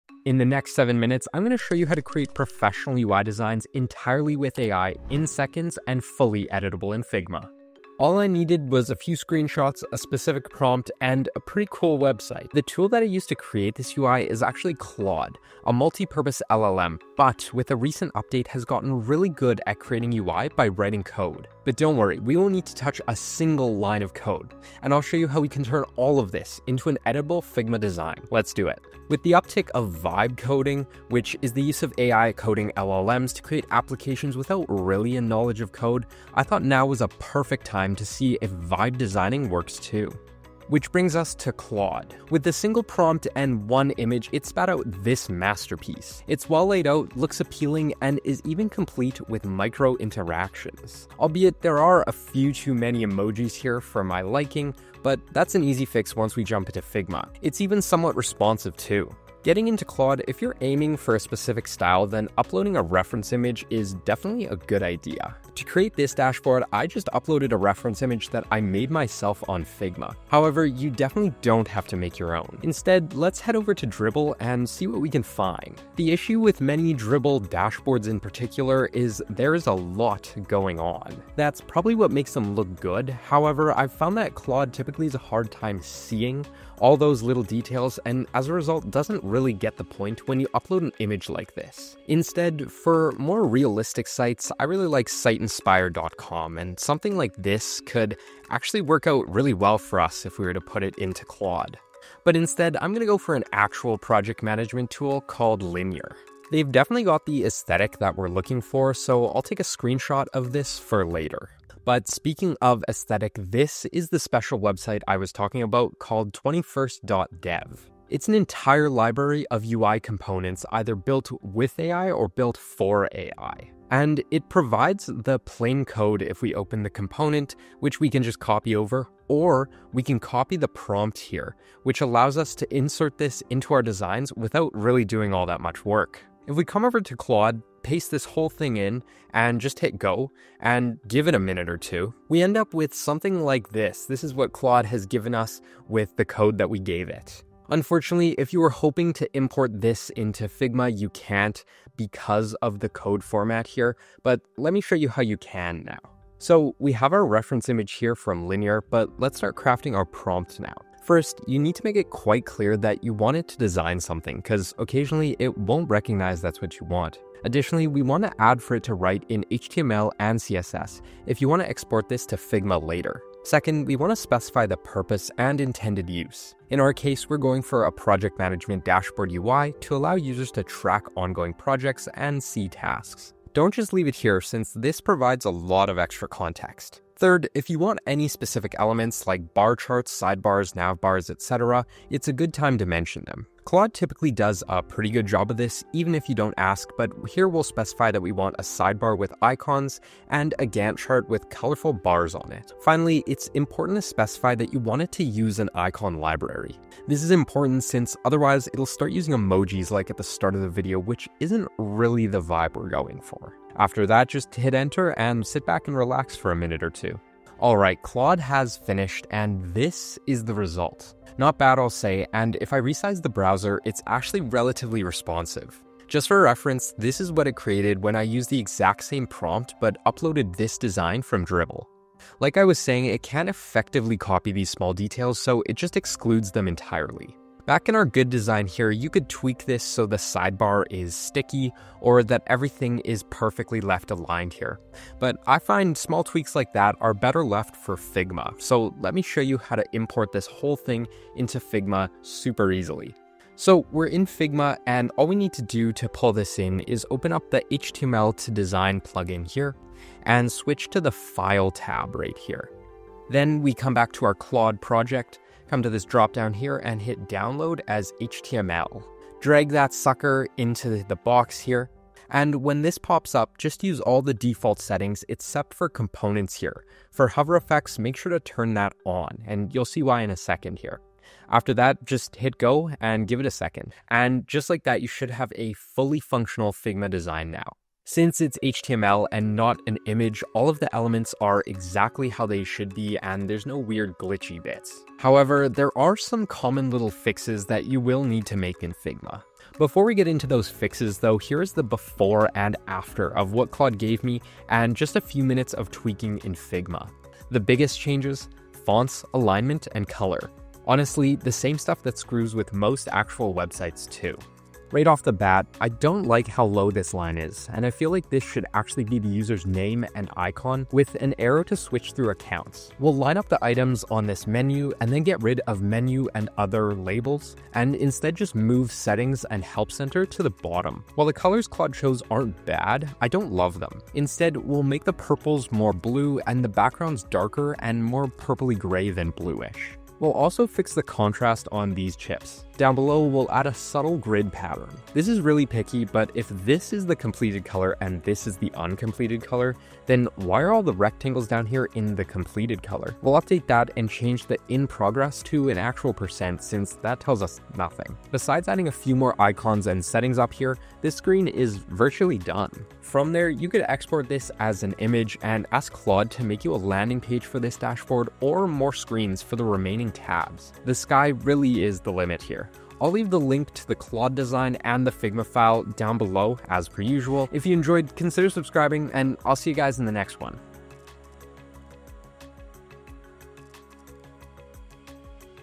Vibe Coding a Pro UI in SECONDS With AI
See how to vibe code your way into well made and interactive UI designs! In this video we used Claude AI to create a project management dashboard. I walk through crafting a prompt, getting the right reference image, and a super cool library full of AI ready components!

Enjoying the videos? Check out some of our other ones!

Timestamps:
0:00 Intro
0:28 Vibe coding
1:03 Reference Images
1:52 21st Dev library
2:35 Prompt crafting
3:37 Results & breakdown
4:55 Common design tweaks
6:11 Outro